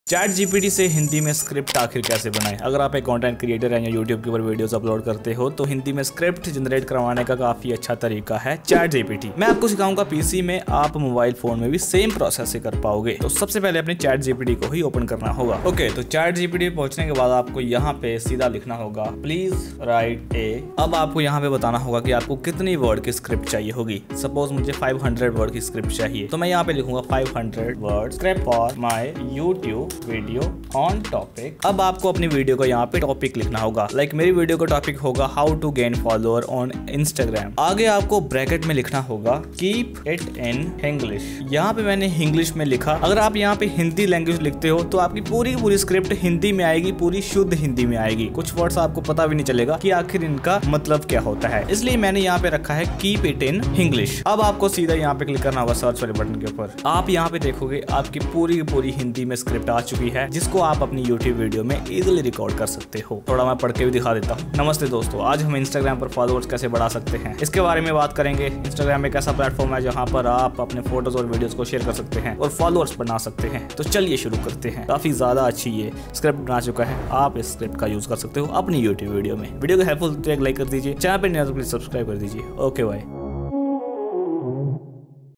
Chat GPT se Hindi mein Script kaise likhe (2023)
In this video, I will Show You How to write a script in Hindi from Chaat Gupta